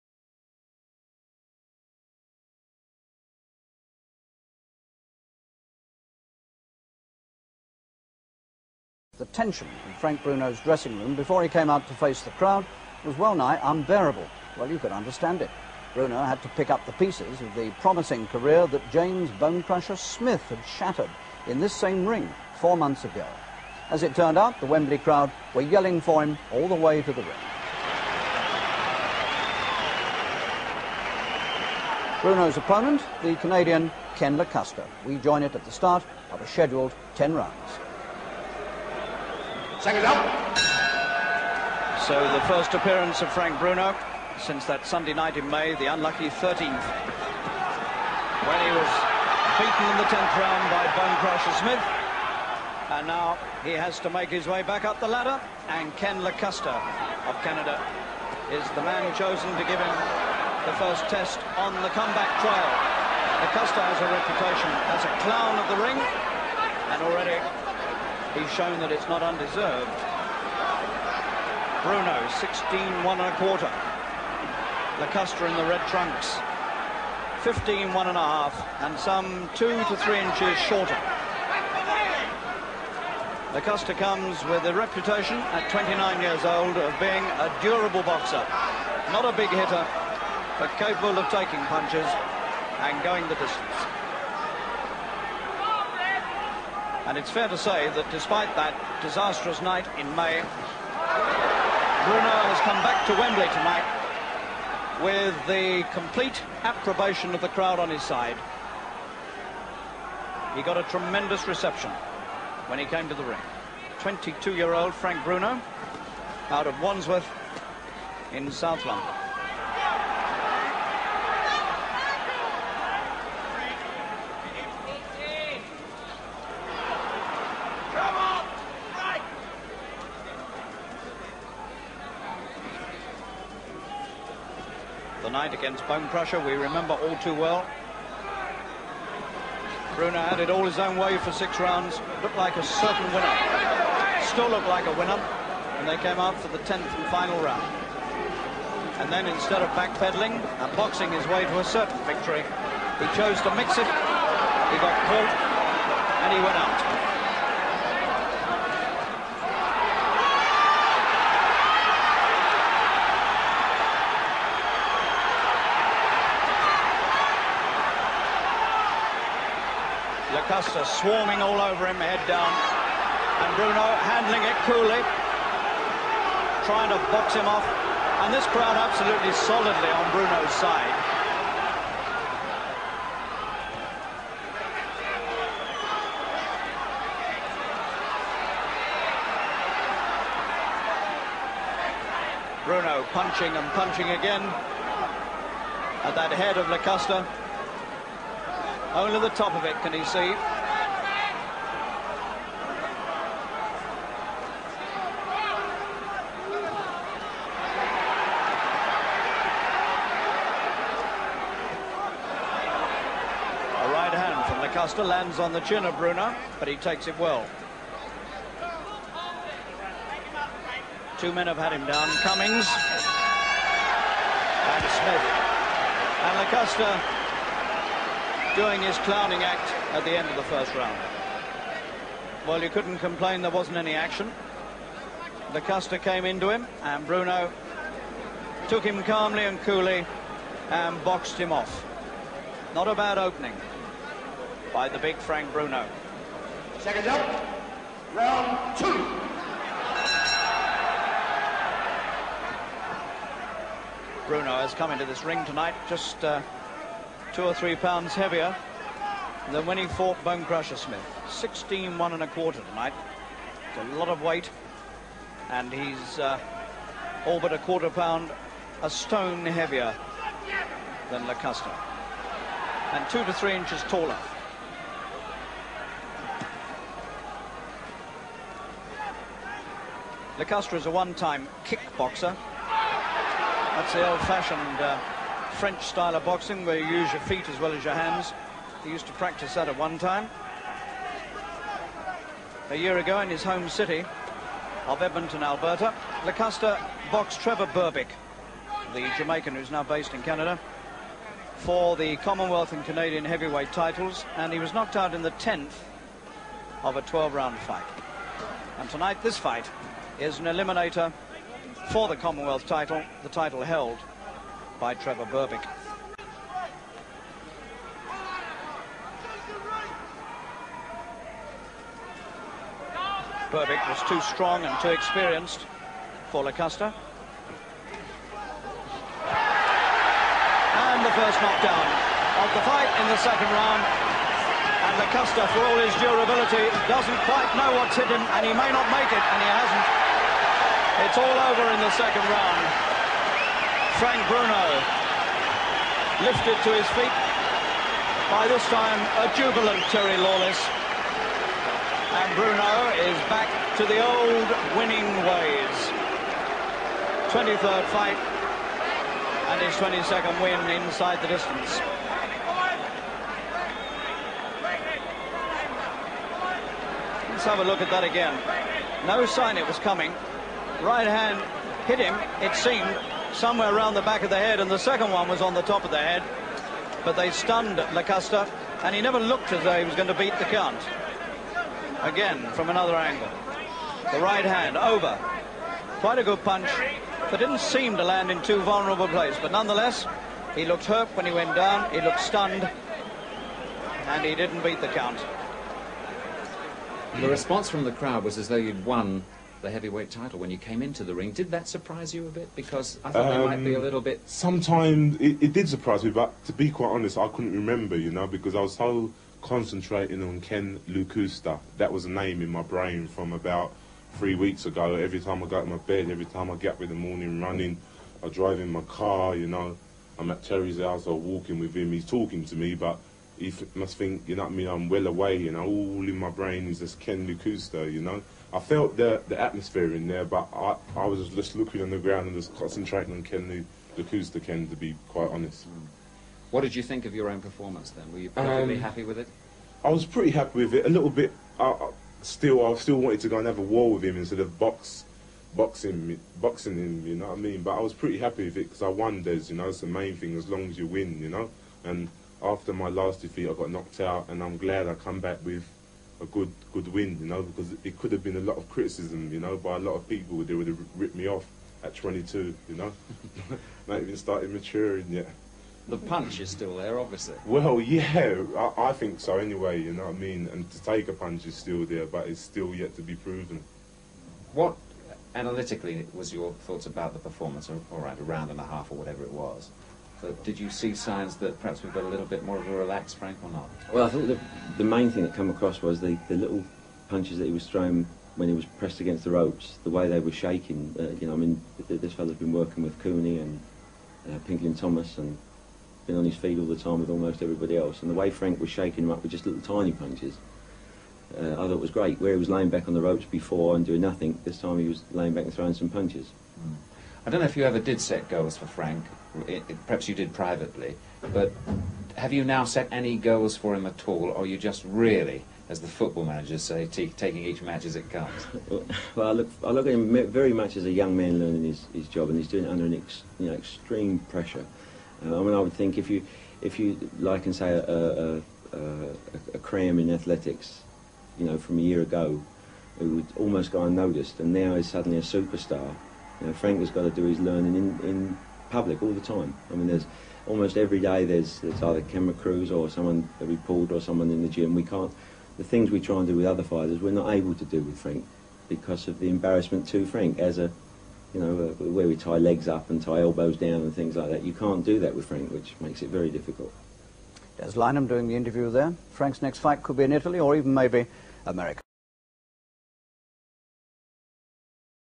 Frank Bruno v Ken Lakusta
25 September 1984 Frank Bruno fought the game Ken Lakusta in a tough looking bout after his defeat to Tim Witherspoon.